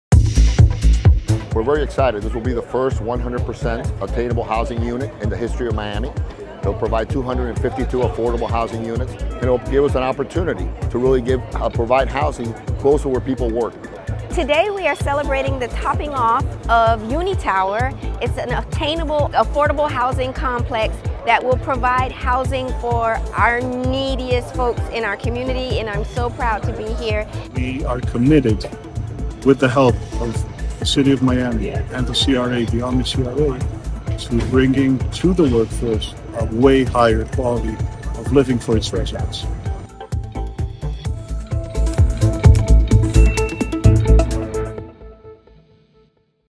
Uni Tower Topping Off: Affordable Housing for Working People in Miami
Unitower is making history as the first 100% attainable housing unit in Miami. With 252 affordable housing units, it offers a housing solution close to where people work. 🌆🏢 Unitower AffordableHousing cityofmiami CommunityDevelopment @alexDLPmiami @IamChristineEsq